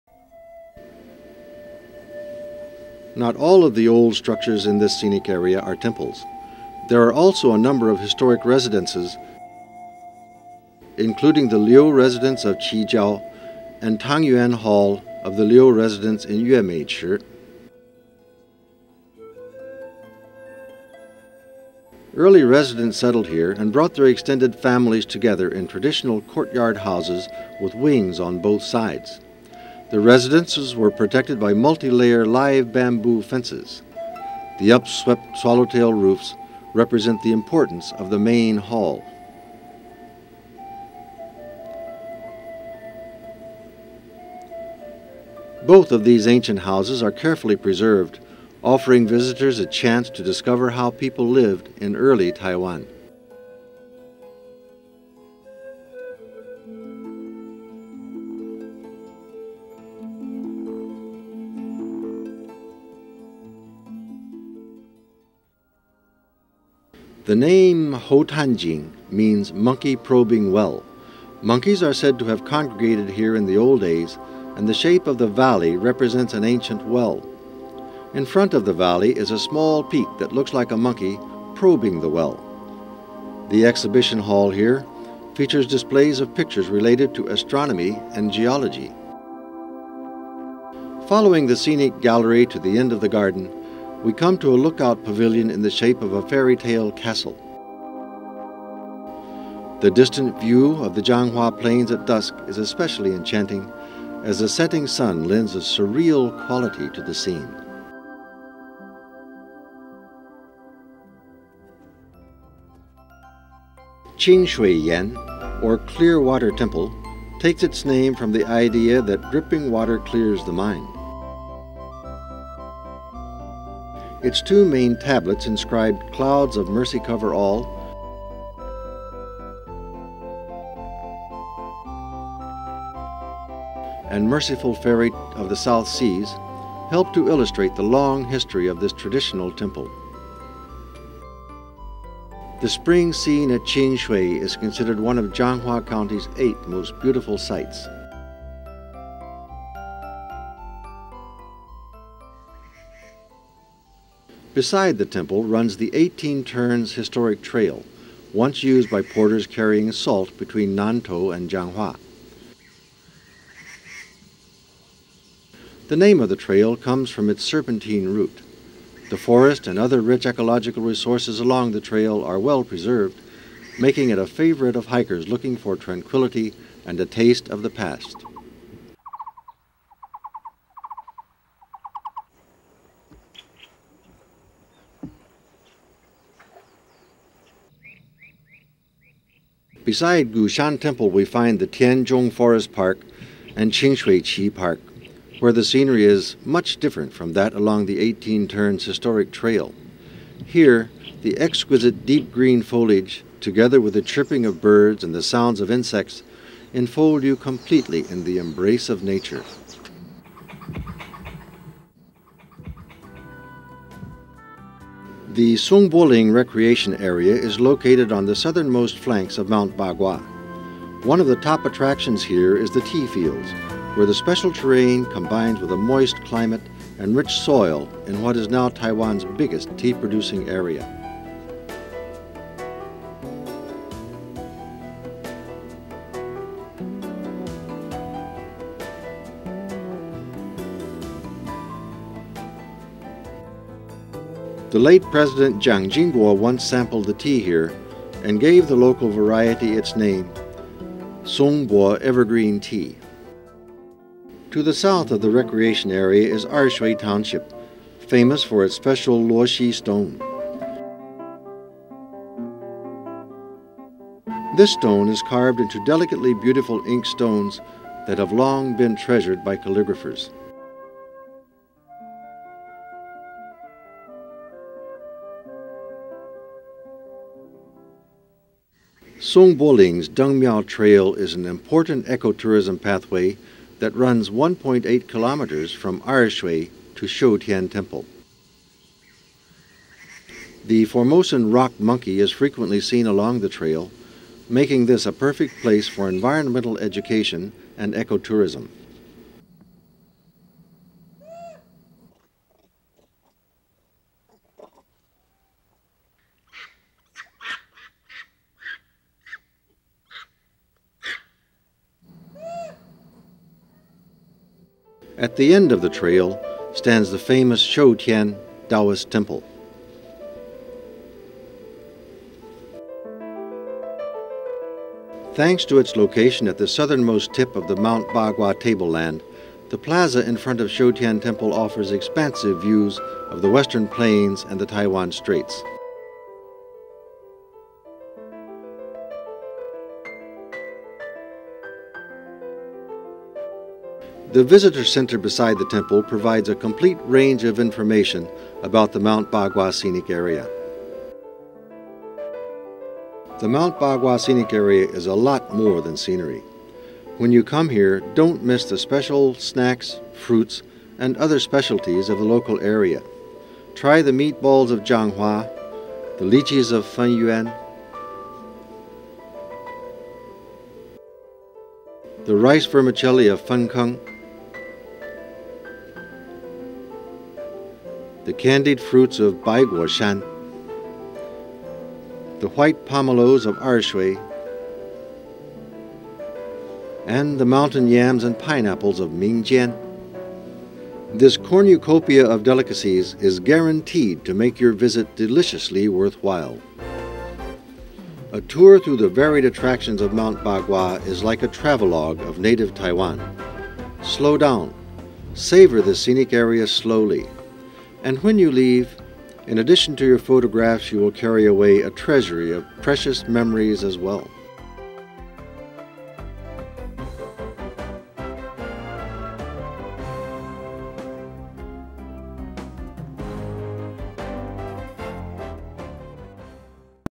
THE ROMANTIC LANDSCAPES LISHAN #1
The Lishan Scenic Area is bisected by the Central Cross-Island Highway and the Dajia River. It is blessed with an abundance of ecological landscapes and natural resources, and with a climate that clearly delineates the four seasons. It is no wonder that Lishan has become a favorite vacation spot in central Taiwan. Just how beautiful is this Little Switzerland? Come with us as we look for the answer.【The information from Tourism Bureau Ministy of Transportation and Communication, Republic of China(Taiwan)】